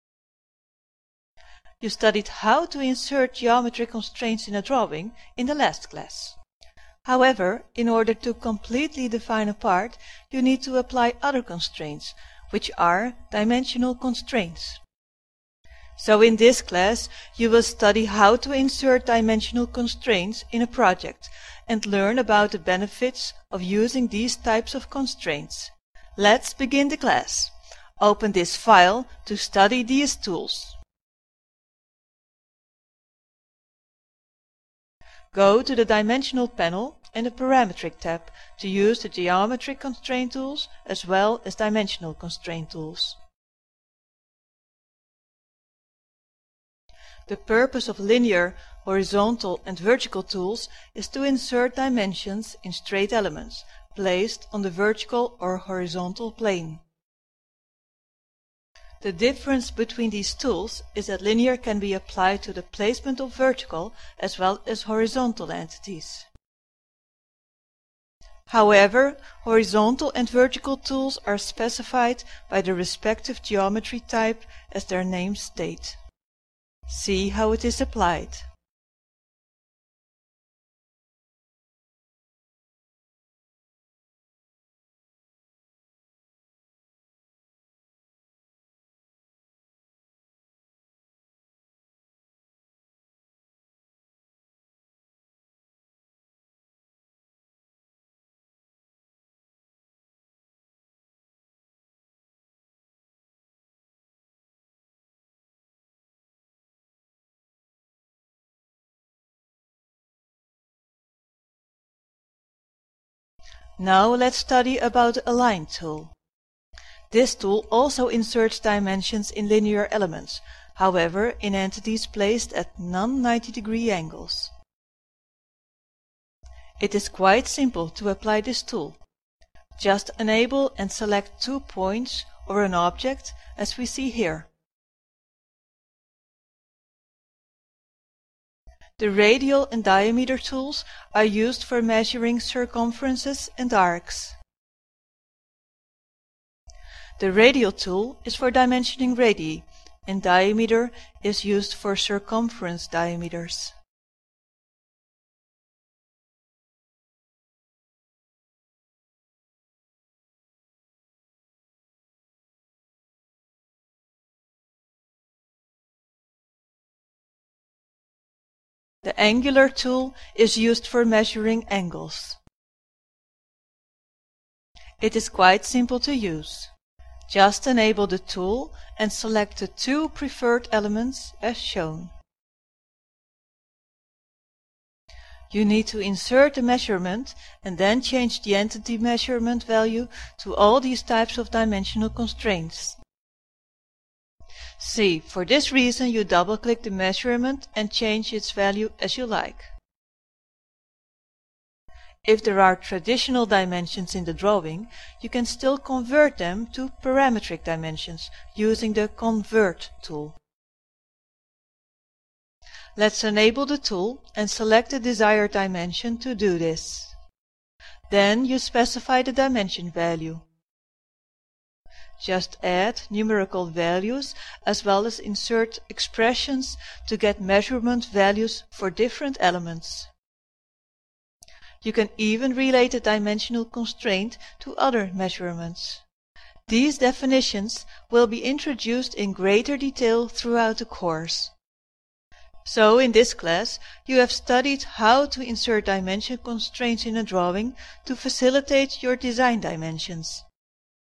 Parametric measurement in AutoCAD - M2A3
Besides the geometric constraints used in a parametric drawing, you also use dimensional constraints to define part geometry. In this class, you will study how to apply this type of constraint to a drawing.